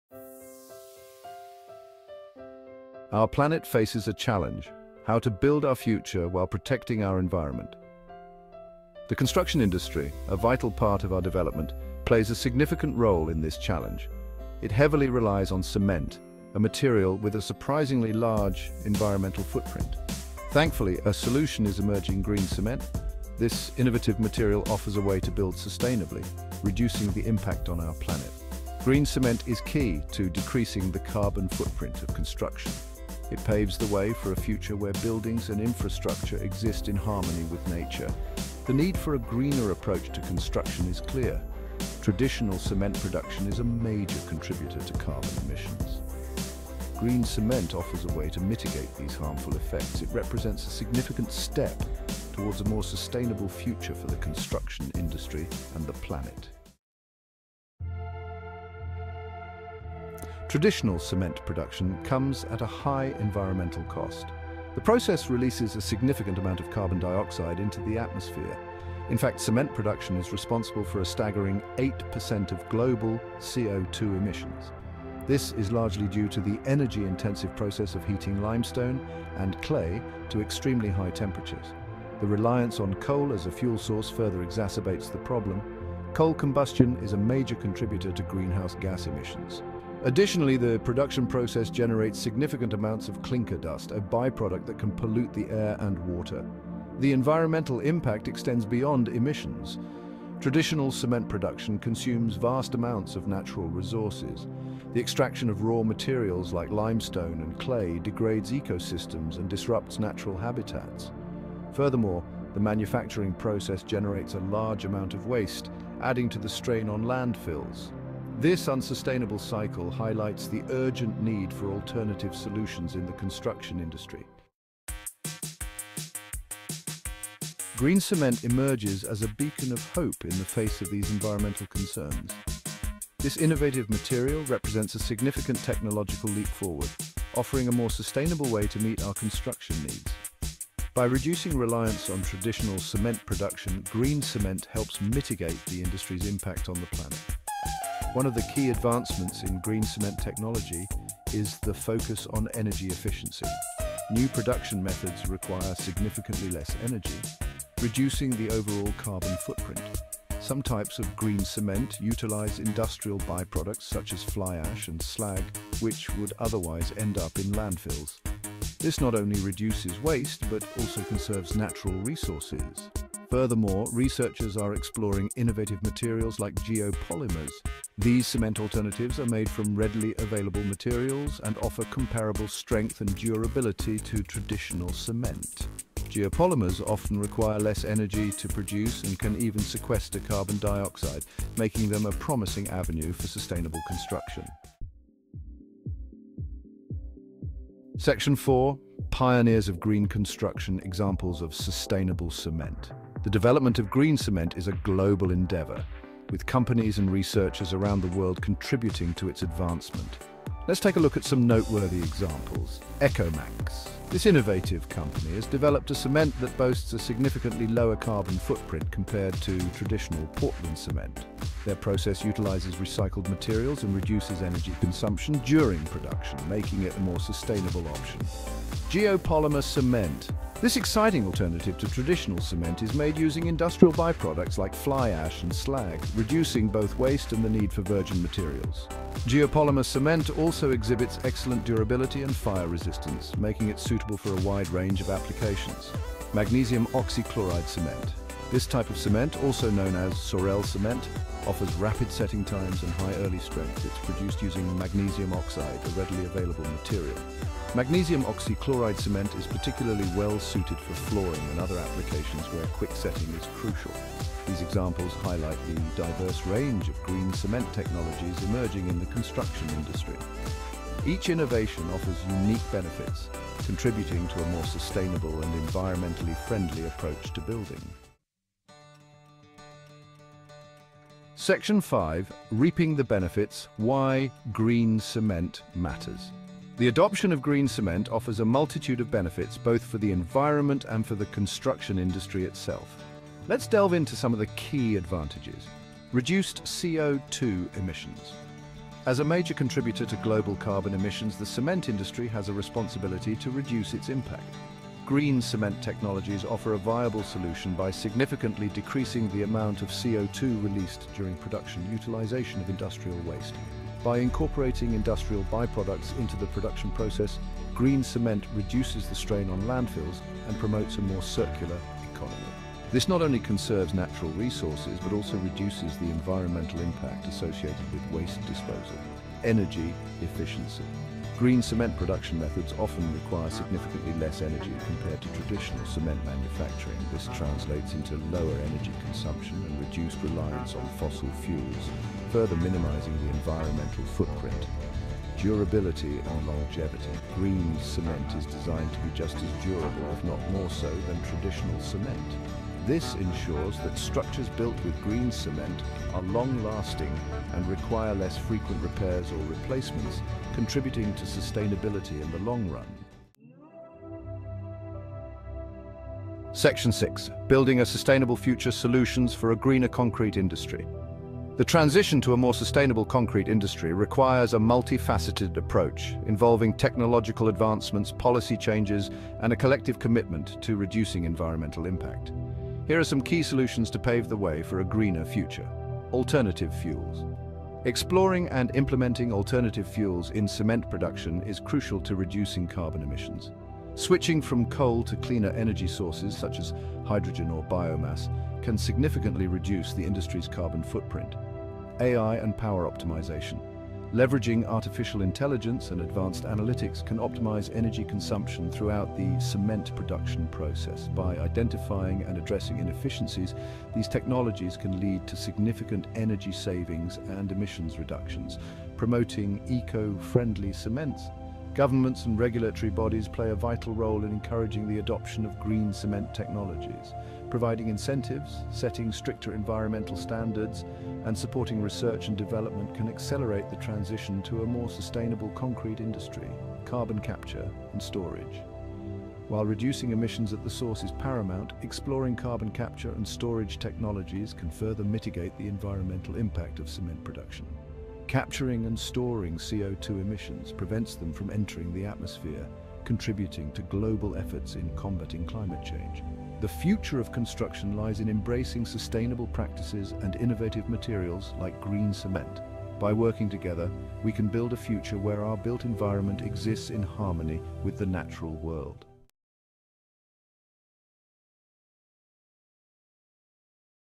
GREEN CEMENT
Context – Green cement as a sustainable construction material
Concept –
Green cement is an environmentally friendly product that minimizes the carbon footprint of cement production.

Why is cement manufacturing polluting ?

India’s cement industry contributes 7% of co2 emission as it is heavily relied on coal. Its main challenges are clinker dust, leaks during production and transport, waste during manufacturing , heavy reliance on coal for power.

How green cement is produced ?

Many attempts have been made to produce green cements and several green cement types have been produced.
The majority of these are based on technological advances that include energy-efficient, low carbon production methods, new cement formulations, geopolymers, carbon-negative cements, and new concrete products.
Additionally, green cement production reduces cement intake, and its major raw materials include discarded industrial wastes like blast furnace slag and fly ash.
Ekkomaxx cement, magnesium oxychloride cement, geopolymer cement, ferrocrete, calcium sulfoaluminate cement, and sequestrated carbon cement are some examples of green cement. Some of these products have been commercialized and used in various projects whereas a number of these cements have not been utilized broadly in construction projects but possess great potential to be used.
It has been estimated that a single tonne of Portland cement releases approximately six percent of the total man-made carbon emissions, which significantly contributes to the current environmental issues. The usage of green cement can provide an effective solution to such pressing complications.

Substituting coal with hydrogen in the cement plant can eliminate CO2 emissions, with hydrogen having six times the calorific value of coal. The cement manufacturer will switch to hydrogen if its adjusted cost, including carbon tax, is equal to or lower than coal costs. There is a demand for green cement, potentially available at a 15% premium.

To make cement manufacturing green focus on 4 areas :

Use alternative fuels like RDF,MSW & Renewable energy, optimise power consumption with AI technologies.
Electrifying kilns to reduce emissions, adopting technologies like rotodynamic heaters.
Developing eco-friendly cements such as EKKOMAXX & ECOPact, using alternative raw materials and increasing fly ash and pond ash .
Improving carbon capture utilization and storage (CCUS) technologies to capture and use CO2 emissions.

Advantages
Lowers carbon dioxide emission as it does not require as much heat during its production, releasing up to 80% less carbon dioxide. Helps to achieve net zero carbon emission target by 2070.
Makes use of industrial waste such as fly ash, silica fume, and last furnace that may require several acres of land to dispose of. As a result, it protects land from becoming a dumping ground and ultimately being destroyed.
Requires less energy since industrial by-products present in green cement, the energy needed in production is greatly reduced. Additionally, it withstands temperature fluctuations and hence decreases costs related to both heating and cooling.
Long-lasting – Having a low shrinkage rate, green cement tends to be more long-lasting than the traditional cement..